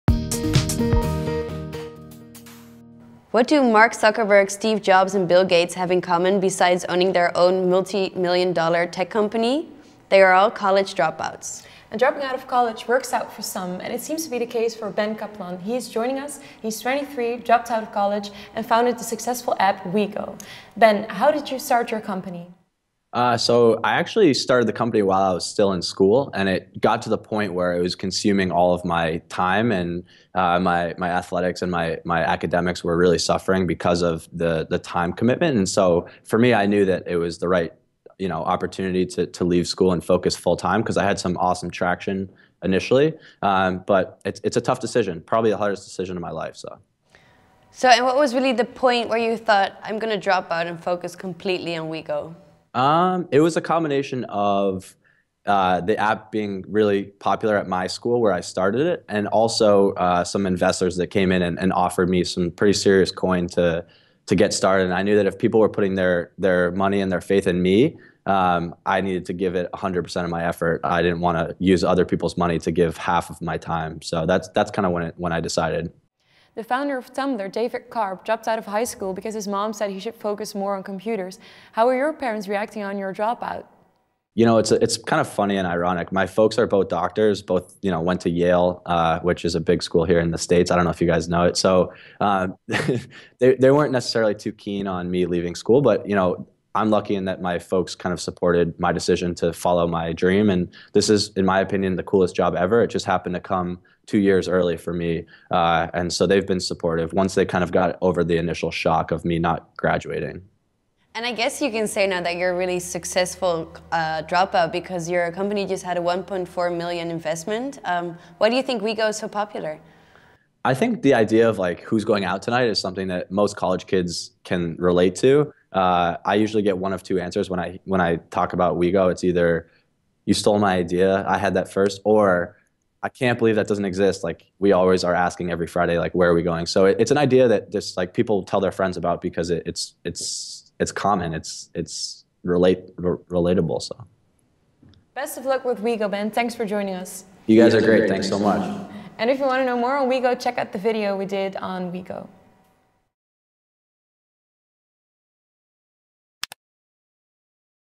Add This Guy To The List Of Successful College Dropouts
Mark Zuckerberg, Oprah Winfrey, Steve Jobs, Brad Pitt, Bill Gates, Lady Gaga, Abraham Lincoln. Just a few names of the list of people who once dropped out of school. However, not finishing their education didn’t stand in their way toward becoming successful and famous. No, we’re not promoting dropping out, but just saying that sometimes pursuing your career could pay off.

It seems to be the case for wonderboy Ben Kaplan. He’s only 23 years old but he already built a 14 million dollar company. In the time that he was still attending college, he found himself and all of his friends asking the same questions every day: where are we going out tonight and who’s going with us? That is why he created the app WiGo: Who Is Going Out.

The app really took off. It’s the hottest thing on many campuses in the US and it got invested in by big names. Since Ben wasn’t able to make class anymore, he dropped out of college to focus completely on WiGo. And he shares his story with IntoConnection today!